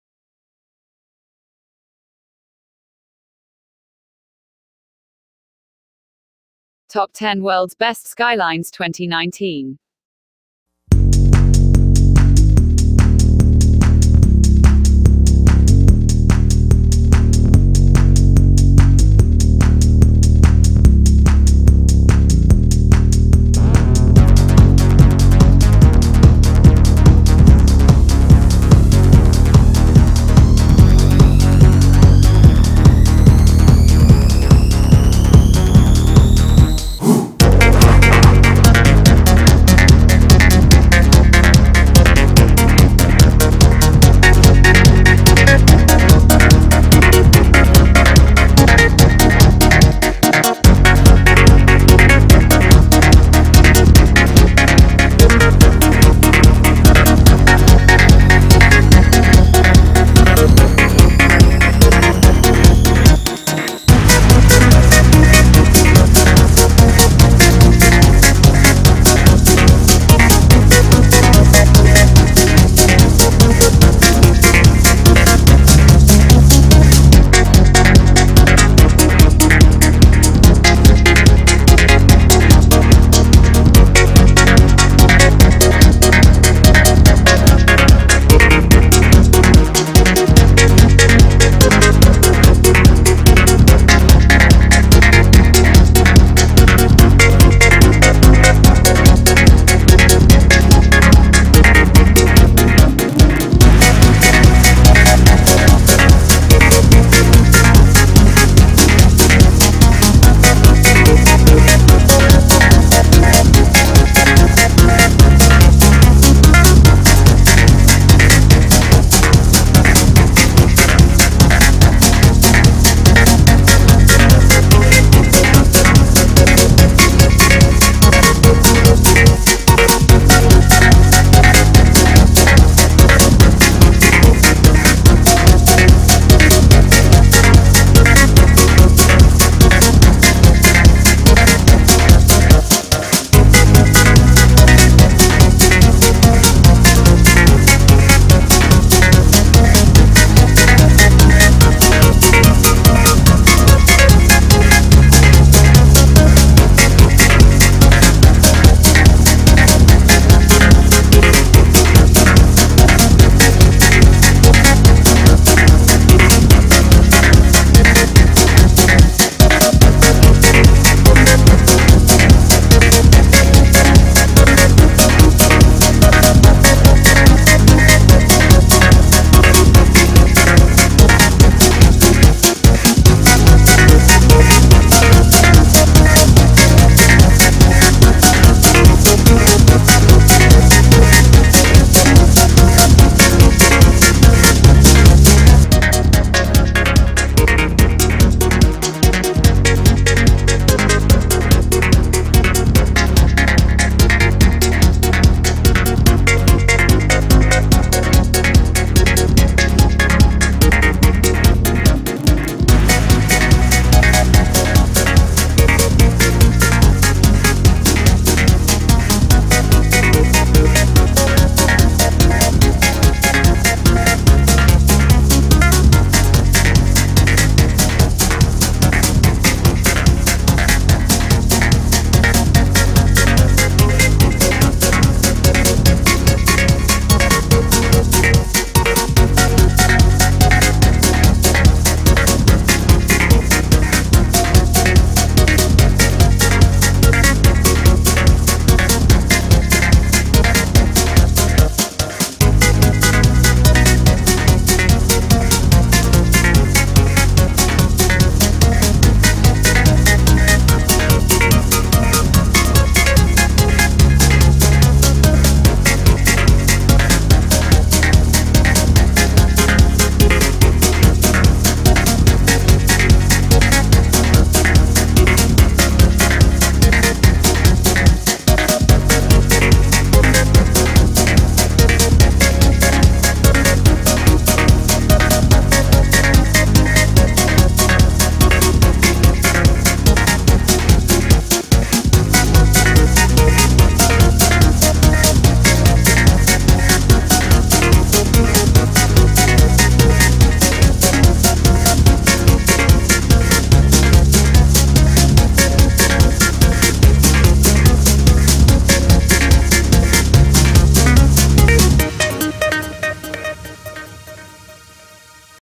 Top 10 World's Best Skylines 2019
1  Dubai
2  New York
3  Hong Kong
4  Shanghai
5  Shenzen
6  Kuala Lumpur
7  Guangzhou
8  Chicago
9  Toronto
10 Singapore